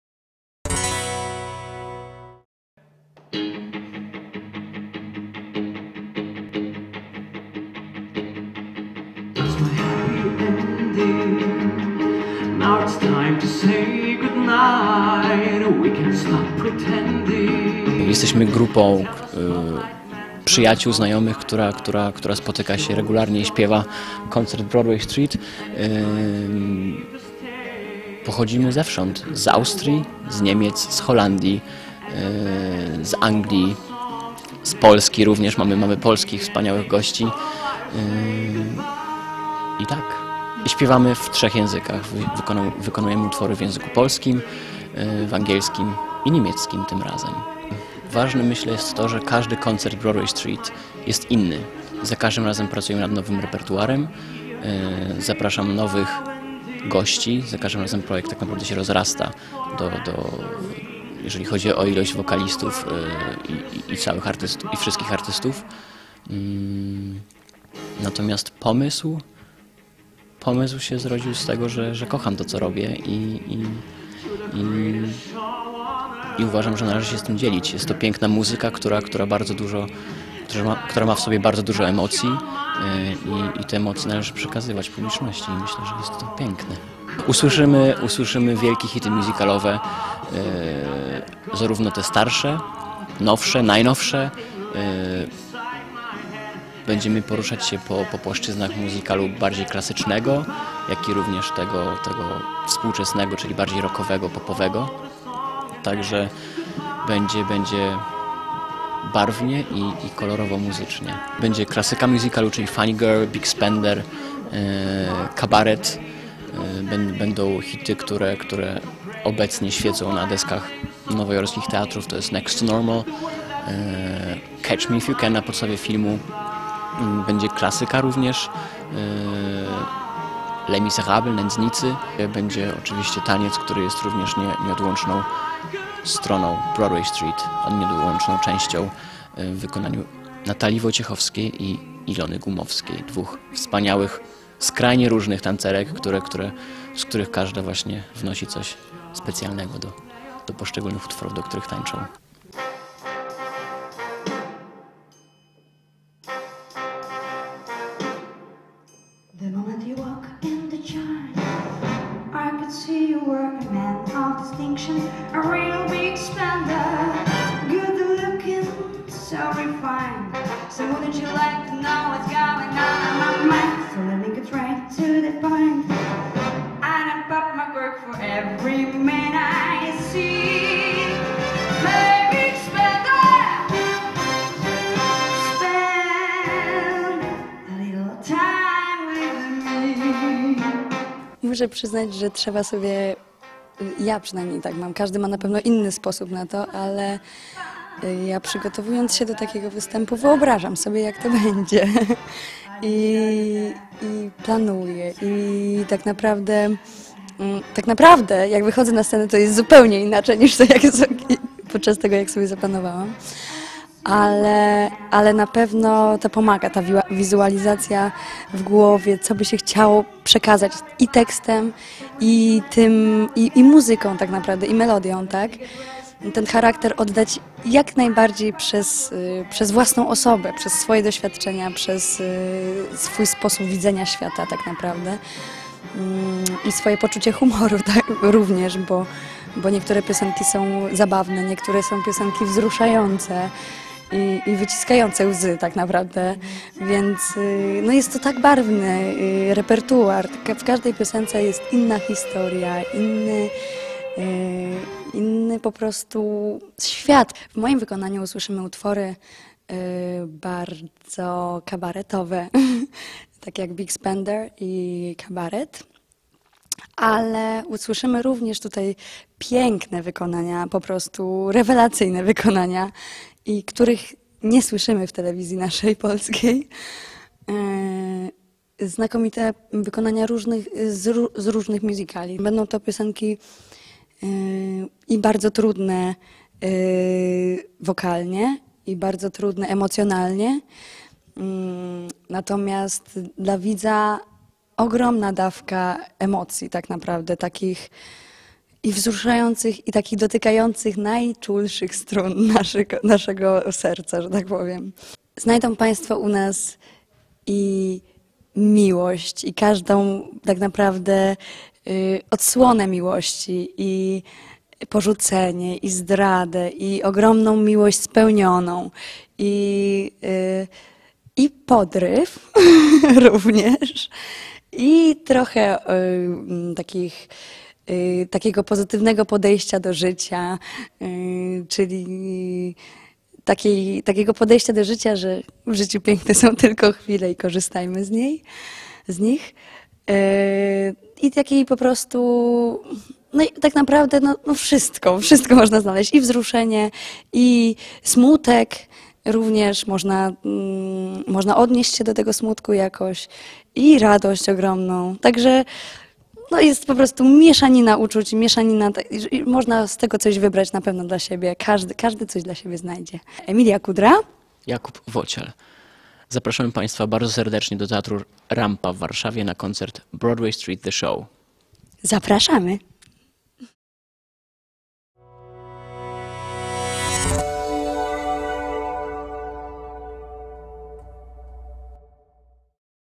Zagraniczne musicale na polskiej scenie
Zobacz więcej na | Przeboje z zagranicznych musicali w trzech językach, międzynarodowa obsada i polska scena. Śpiew, taniec, gra świateł i cieni. To właśnie "Broadway Street - the Show!" w Teatrze Rampa.